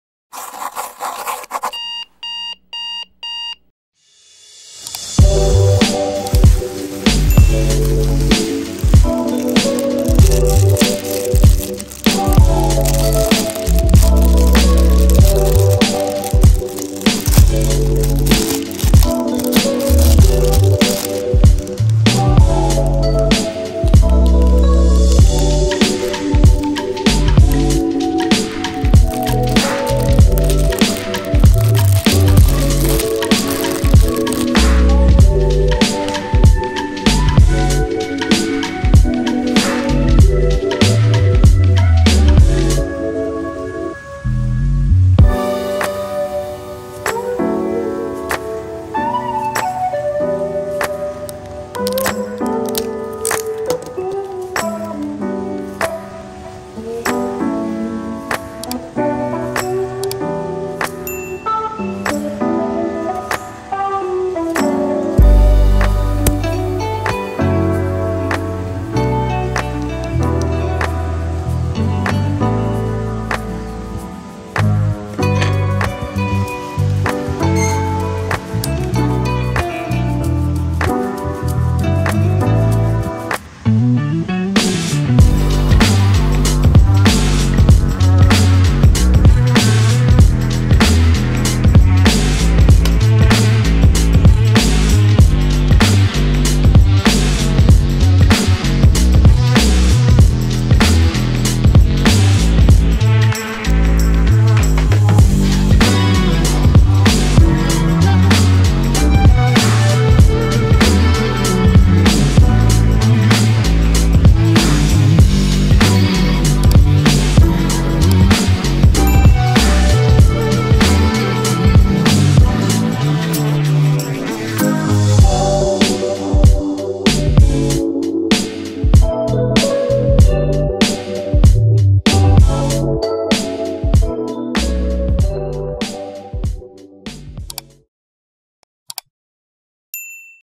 MOTOR HONDA TACT KECIL BANGET MIRIP ASLINYA
Beberapa waktu lalu membeli mainan disalah satu akun toko online sebuah motor berskala 1/32 dengan jenis merk motor honda tact.